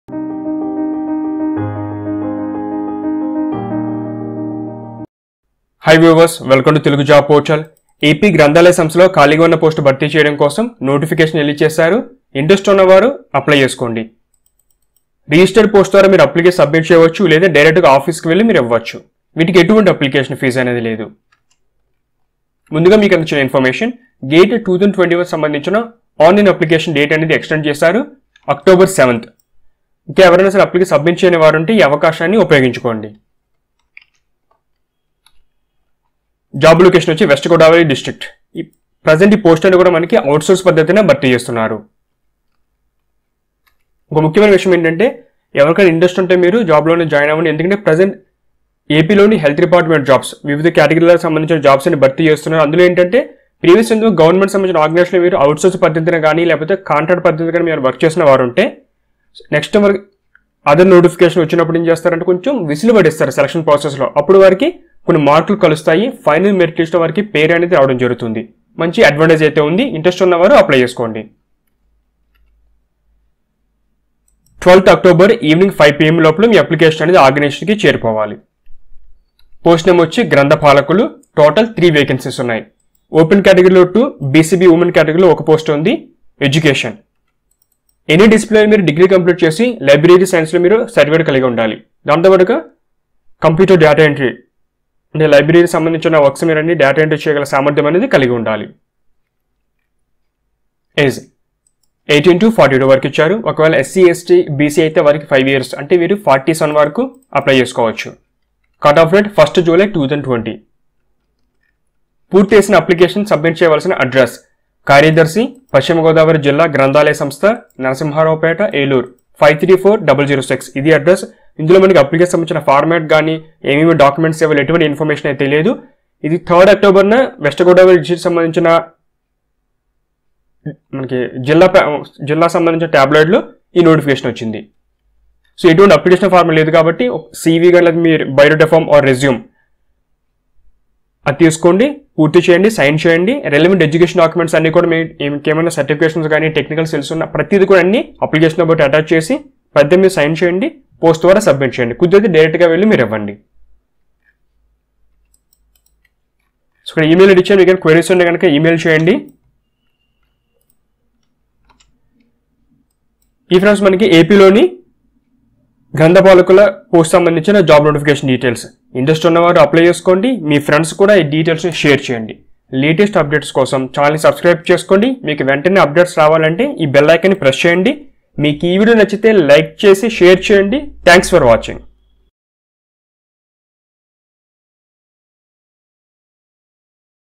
AP Library Recruitment 2020 | AP Library Jobs 2020 | AP Library Department Jobs 2020 | Telugu Job
Andhra Pradesh West Godavari district invites applications for the posts of grantha paalakulu. Interested candidates submit the application directly to the office or through registered post.